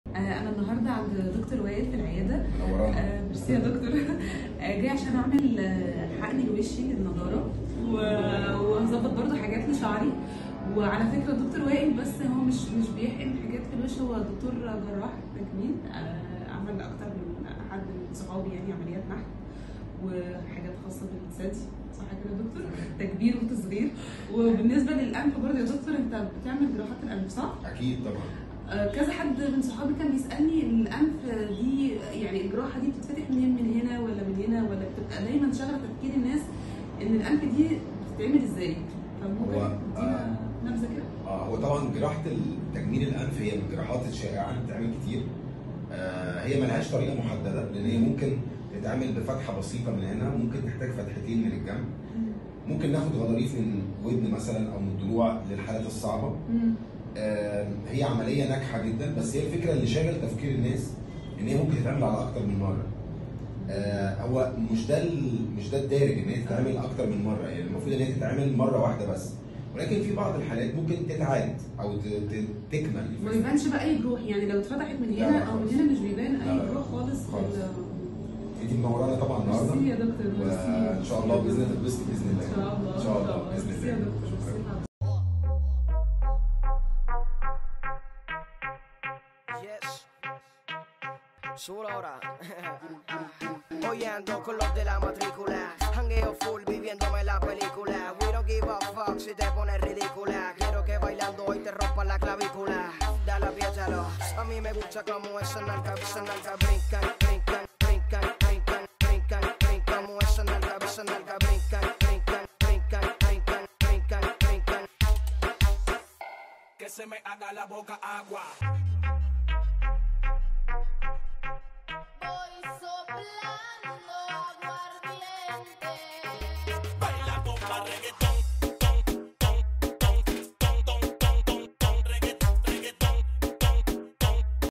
حقن الفيلر بالتفصيل و بدون ألم.. مع ا.د.وائل غانم
نتيجة طبيعية و إطلالة مميزة للبشرة ..بعد حـ.قن الفـ.يلر مع ا.د.وائل غانم👇
انتظروا النتيجة النهائية لصديقتنا😁👌
دكتور_وائل_غانم#